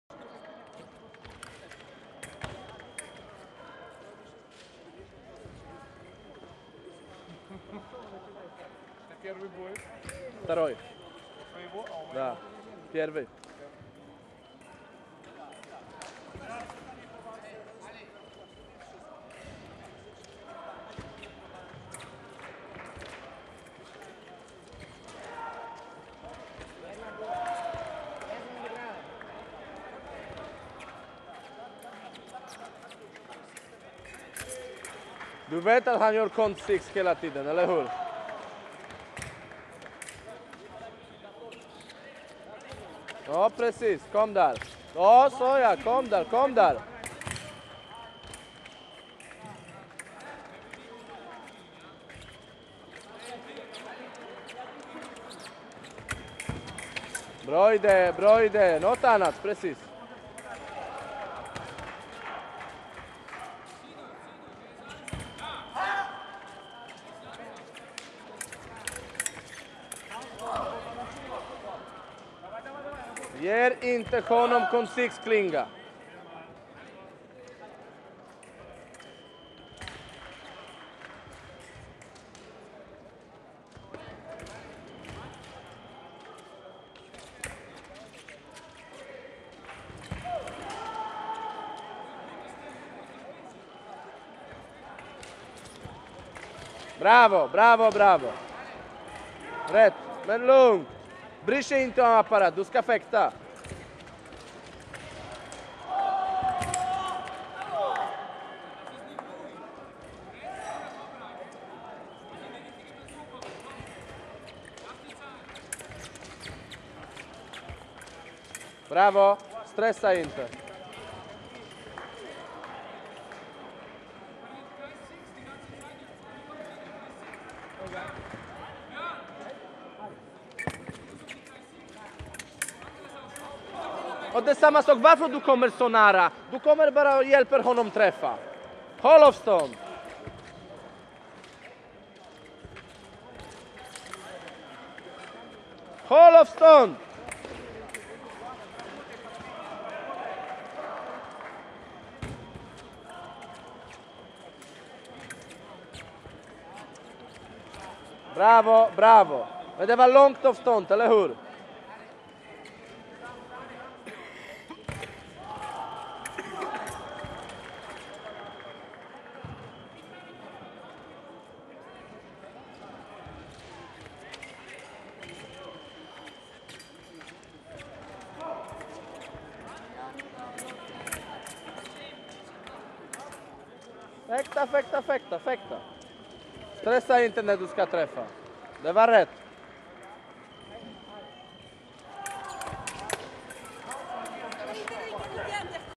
Cadett EM Porec 2012 FH Lind af HAgeby 1.avi
Cadett European Championships Porec 2012, Foil men, Poule, Adrian Lind af Hageby EGF, SWE.